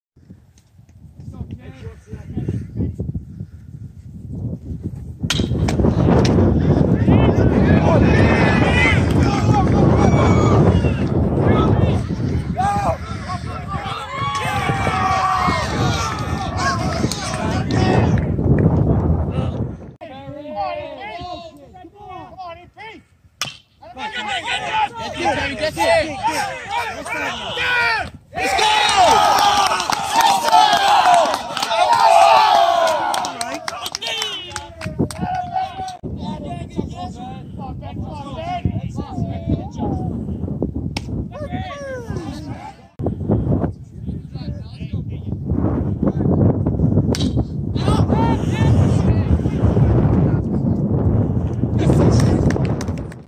Apponequet vs GNB Voc-Tech baseball
Lakers win 5-4 on April 10, 2019